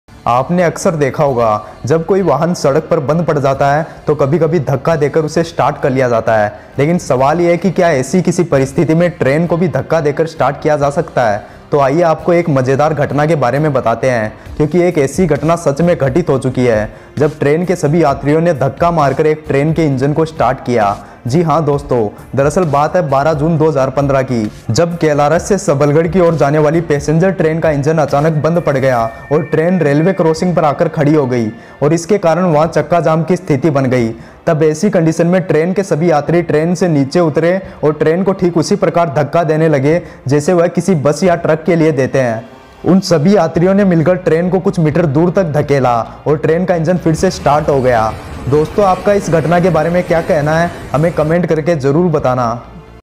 लोगों ने Train को भी किया धक्का Start | Amazing Train Incident | Train Railway
Voice Over:- Harish Prajapat
Scripting & Editing:- Kamal Saini